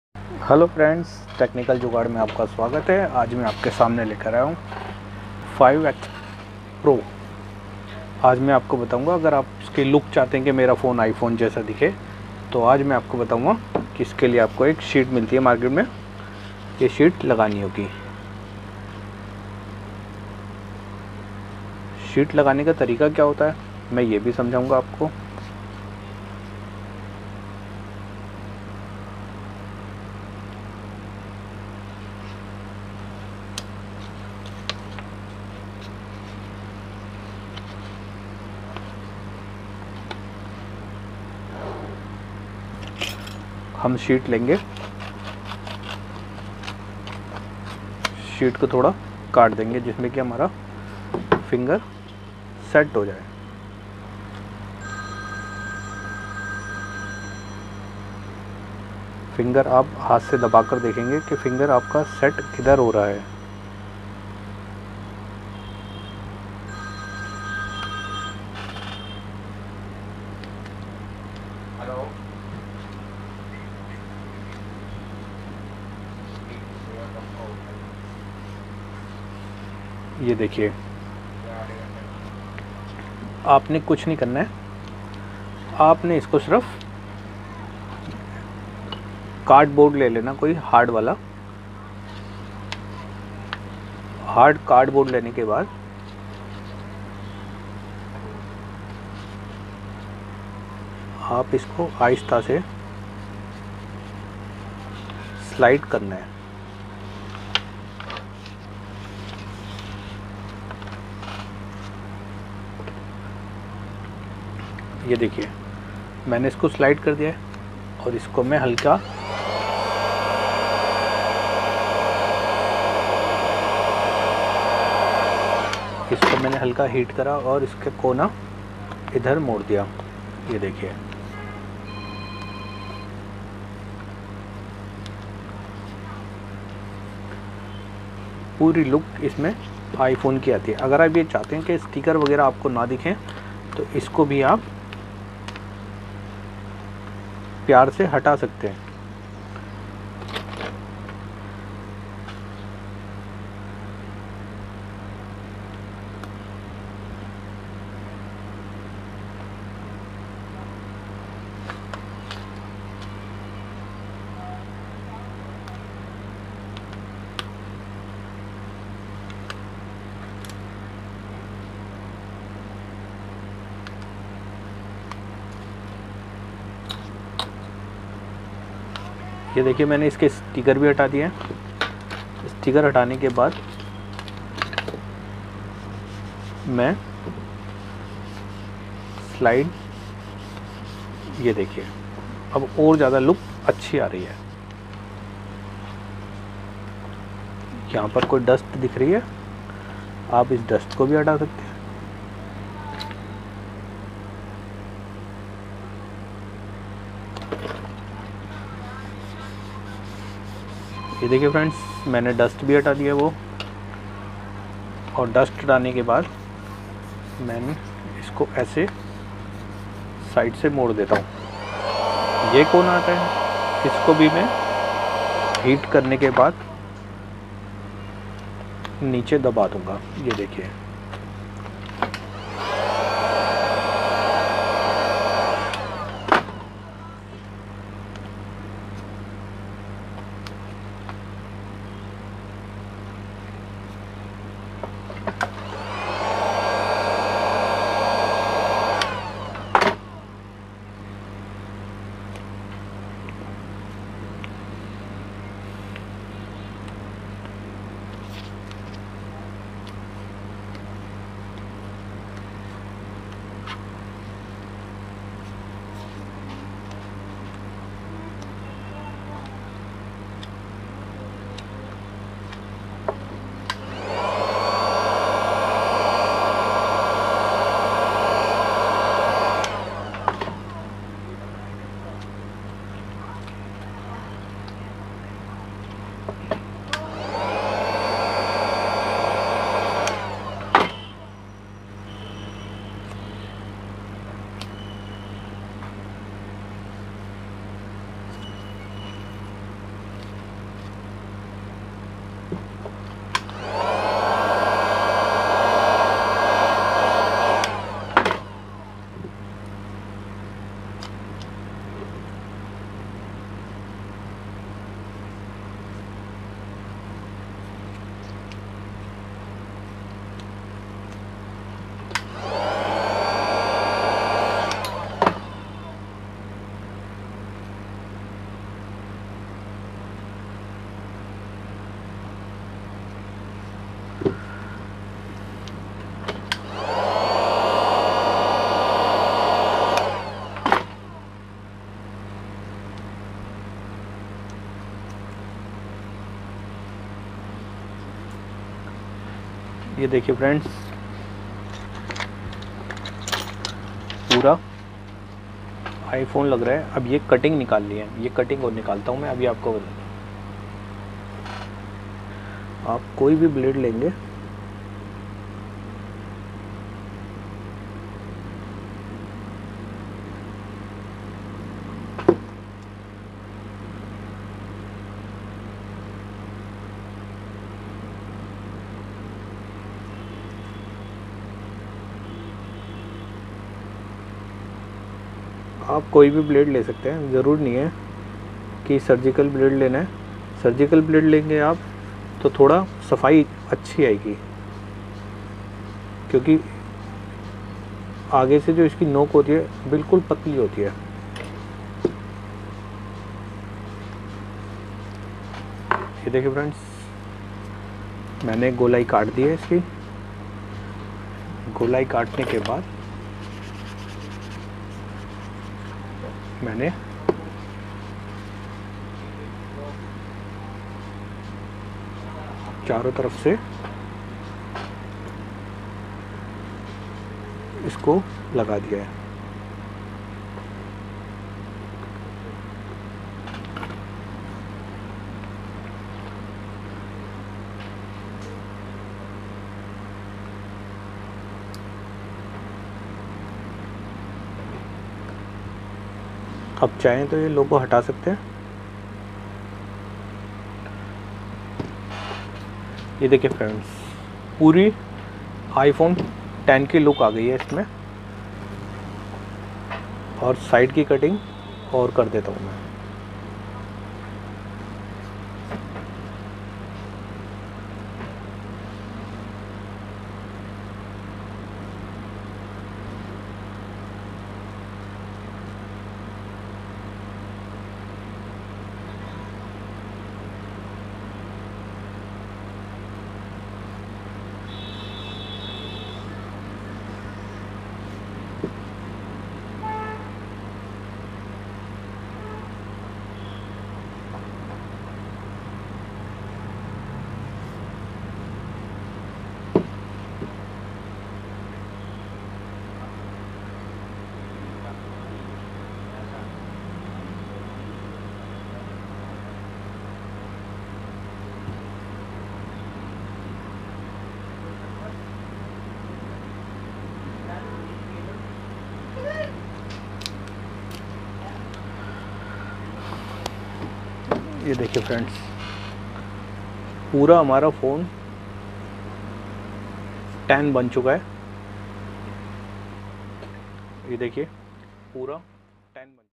Redmi Note 5 Pro Converted in iPhone x With apple Lamination Trick for Any Mobile Phone New gadget
Hello friends is video ko dekh kar aap apna phone iPhone X main convert kar sakte hain skin Laga kar

lamination
New gadget Nagri